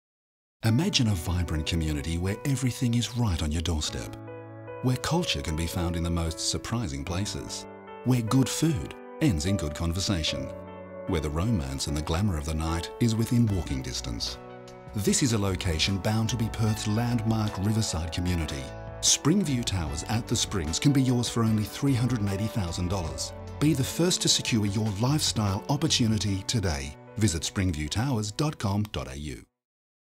Spring View Towers by Finbar - Commercial 2
Spring View Towers offers a new and exciting residence in Perth. The entertainment, shopping and dining options are endless. The CBD is just a short six kilometre drive or why not stroll along the Swan River foreshore, take in the excitement of the Crown Perth or enjoy the coffee culture of the bustling Victoria Park's cafe and shopping strip.